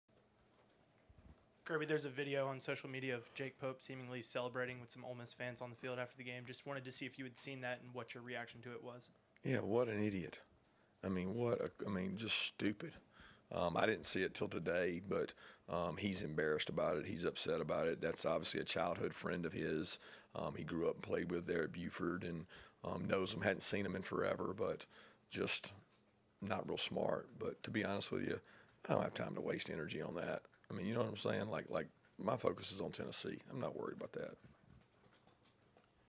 'What an idiot' says Kirby Smart about UGA player caught celebrating with Ole Miss fans
The Georgia head coach was candid about how he felt regarding the actions of one of his players after UGA's loss to Ole Miss.